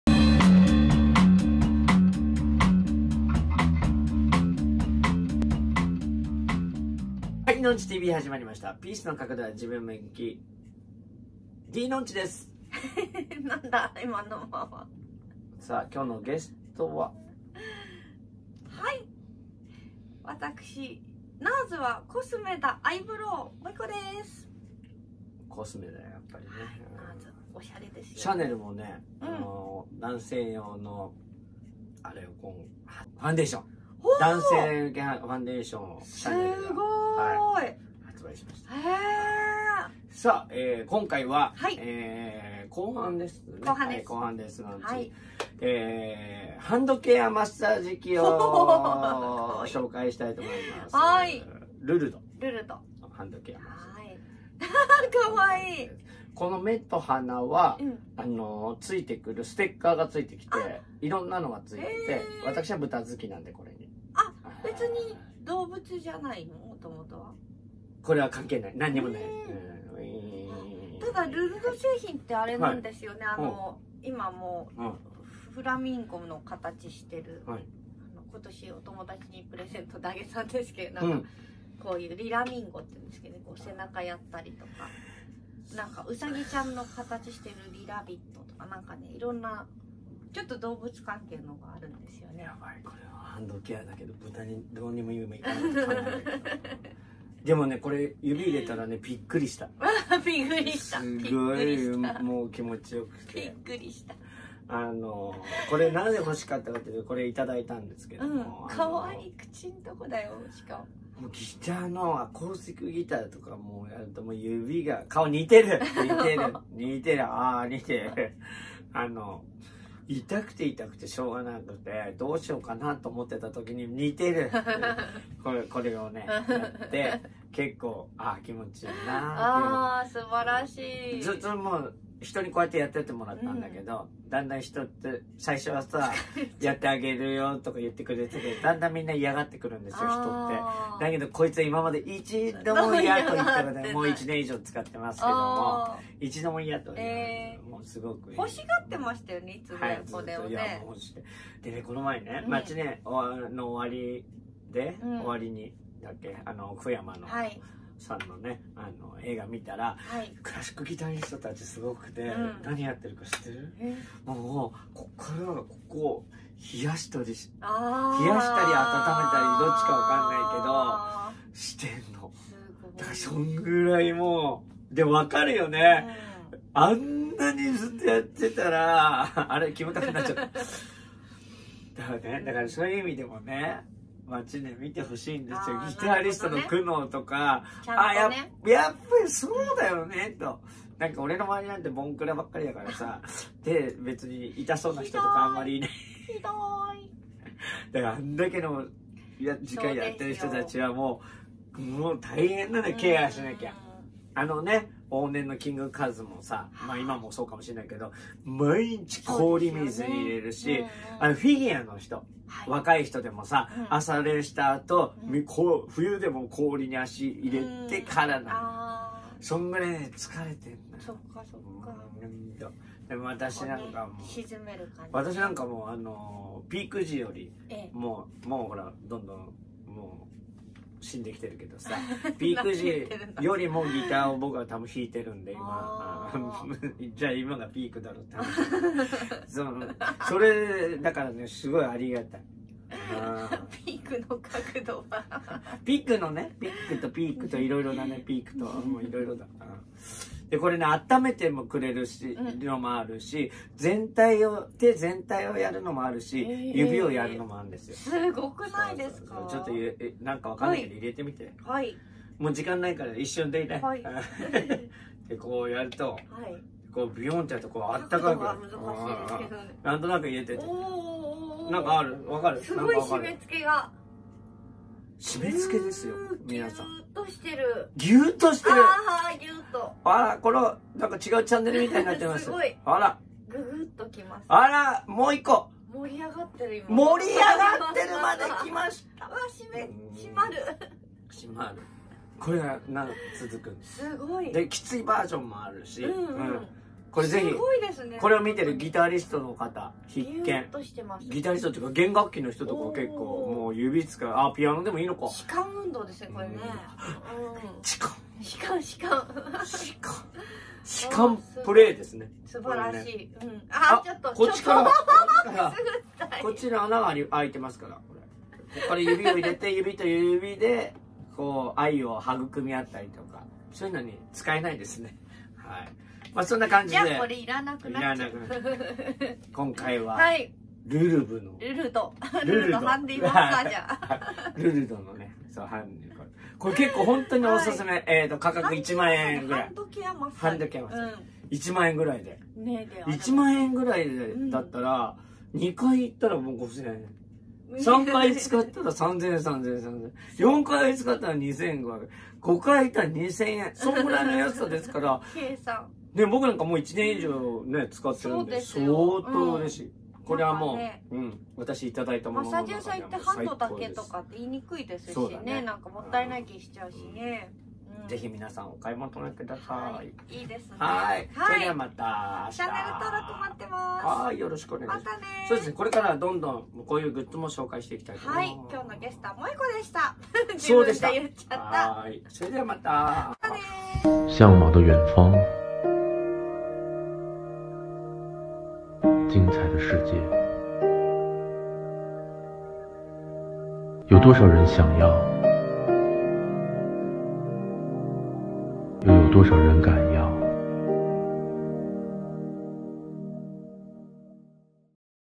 ハンドケアマッサージ。ルルド。ギターの後にどうぞ！のんちともえこ
ハンドケアマッサージ
ルルド

ゲストは網戸もえさんです。

のんちファンクラブ
『おふぁん』第1期開始。